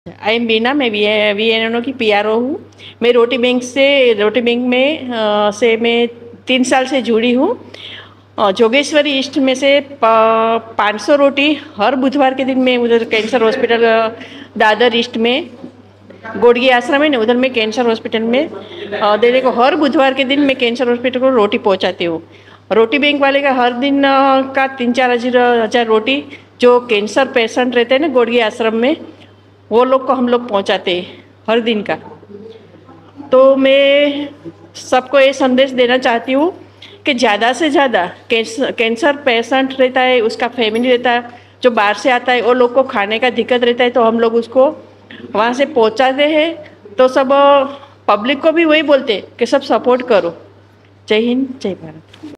Governor of Maharashtra Shri Ramesh Bais Presides I inspire awards organised by PMk Foundation
On the International Women's Day, Hon’ble Governor of Maharashtra Shri Ramesh Bais Presides over 2nd I Inspire Awards for Cancer Warriors and Cancer EcosystemOrganize By PMK Foundation